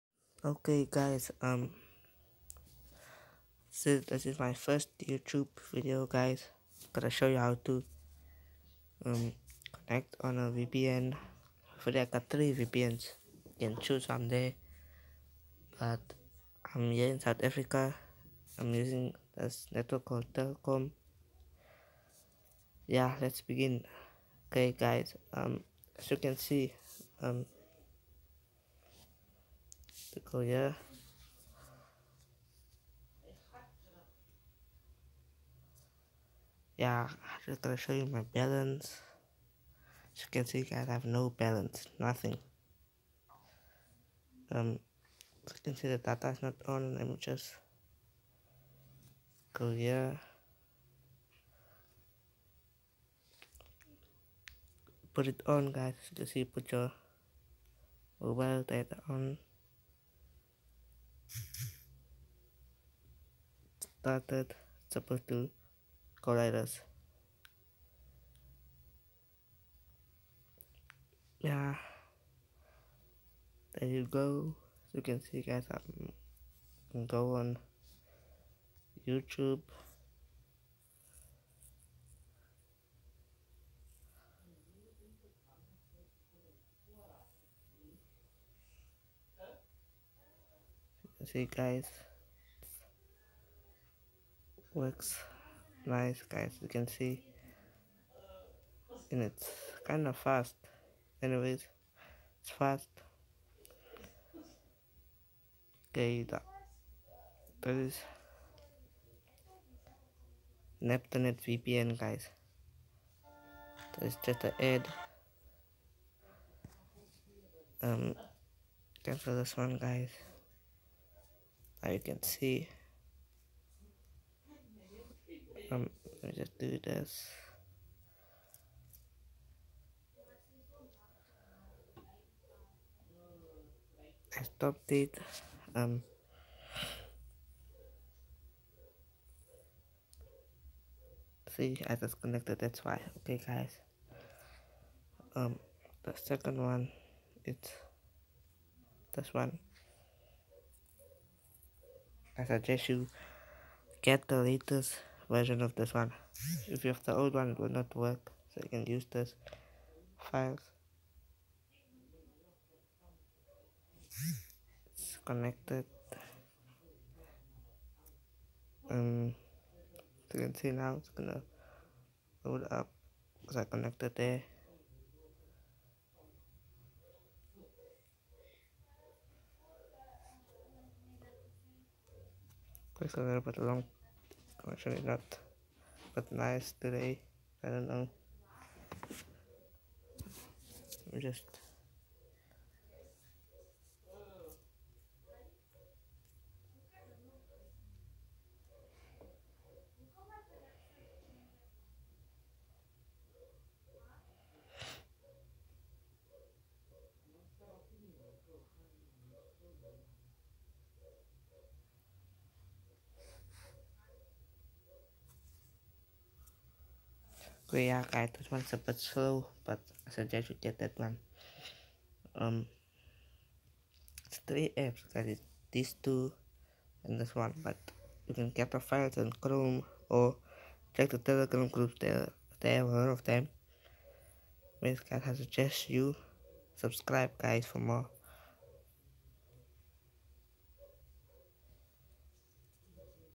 how to connect to free vpn telkom
today im gonna show u how to get free data from a vpn in south africa maby it will work on other countries

ha tunnel vpn in playstore
nepster net vpn playstore

thnx